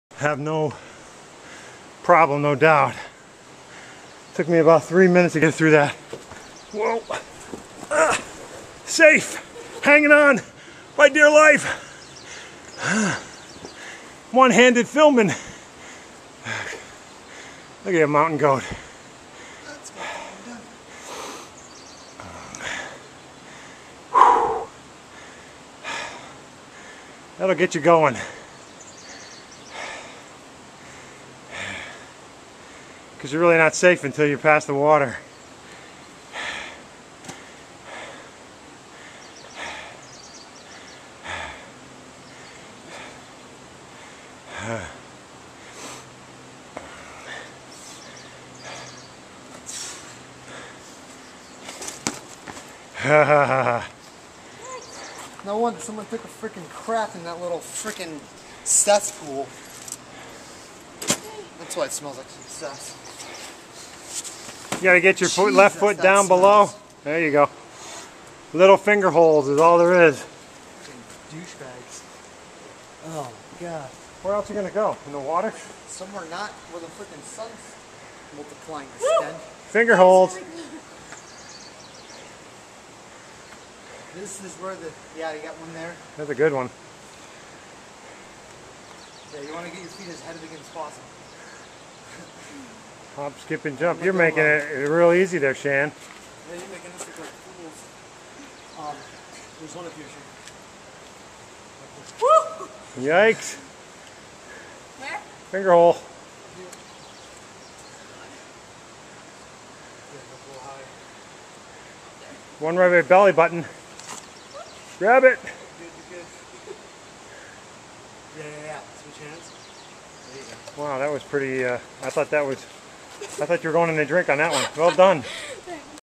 Malibu Creek SP Traversing area #2
This pool is the second area you have to traverse to get to some of the climbing areas within the park.  Traversing the pools are great novice rock climbing places because if you fall, you fall into the water and they are not high off the ground.